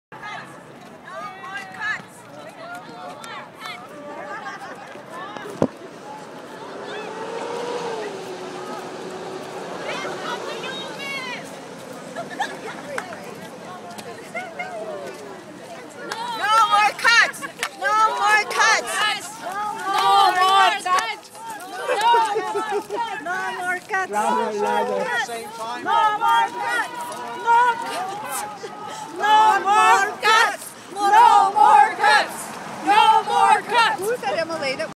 AUPE Rally to Protest Covenant Health's Workforce Realignment
Seniors care workers held a rally outside the Edmonton General Hospital on Wednesday, Dec. 10 to protest Covenant Health's workforce realignment, which will impact 650 staff at three Edmonton area seniors facilities, including Youville Home. General support workers at Youville Home belong to the Alberta Union of Provincial Employees, which organized the rally Wednesday. Health care aides, licensed practical nurses and therapeutic staff at the other Edmonton sites belong to AUPE. They have filed grievances with Covenant Health.